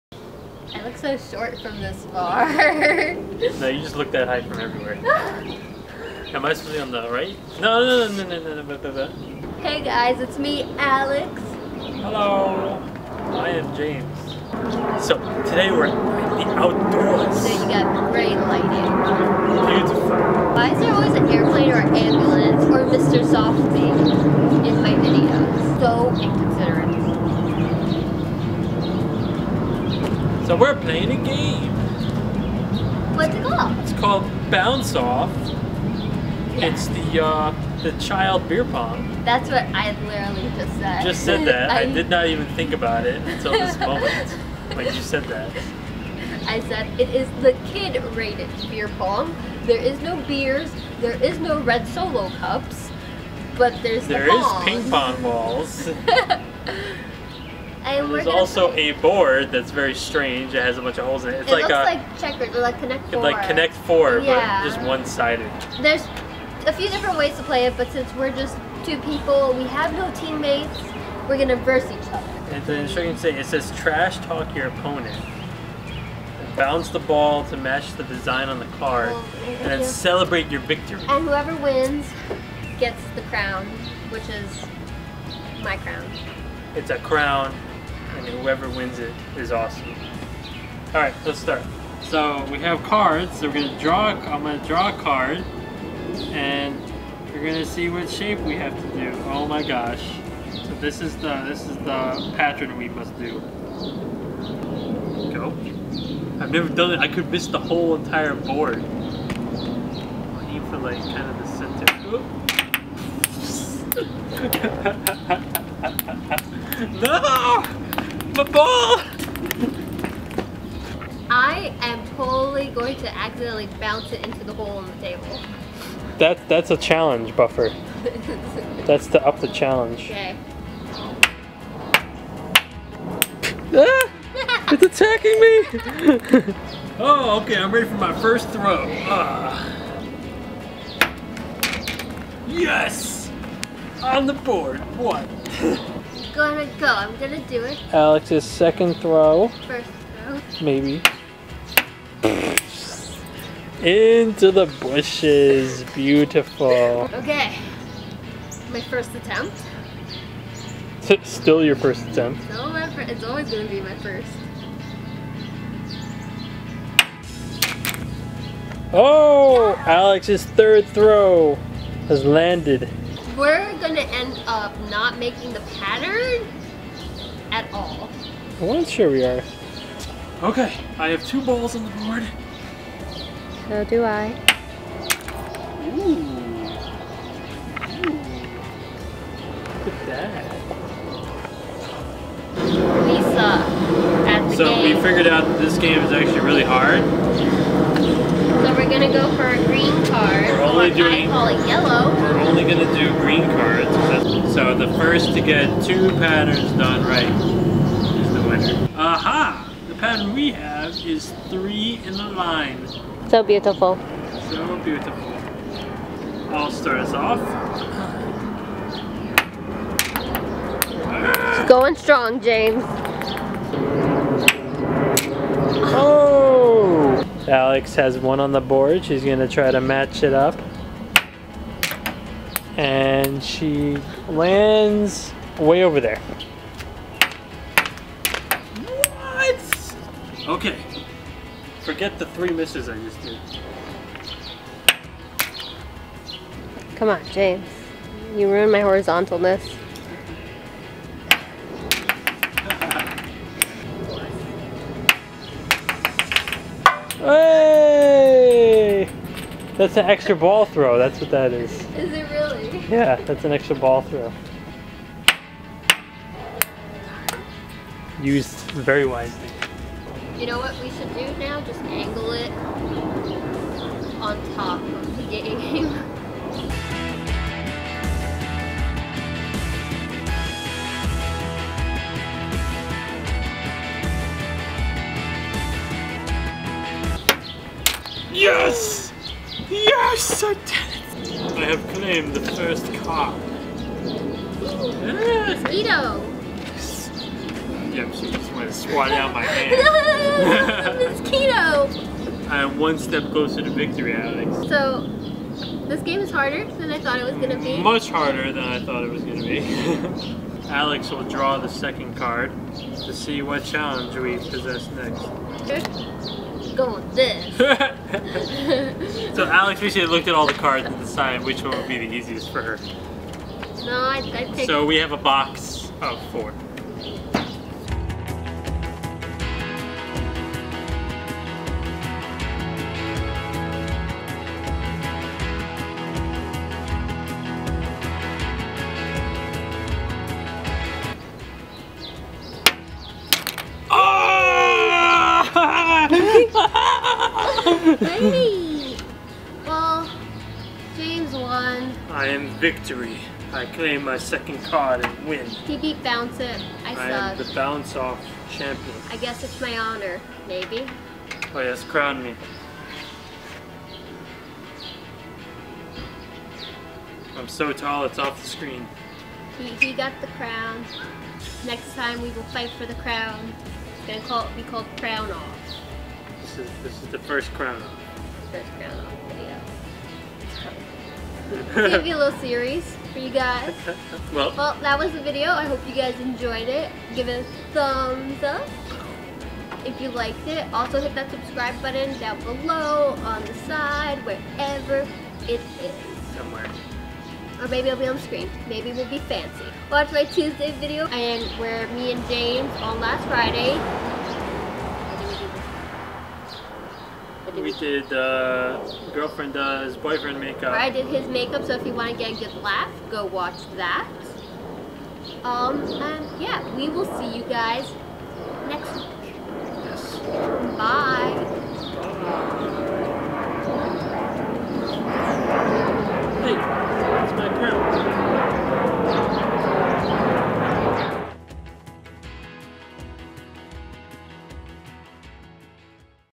Let's Play - Bounce Off! |SimplyAlex♡
This is a new way we are taking on challenges, whoever wins get the crown. It is easier to rub it in each others faces that way. Let us know what you think about the crown!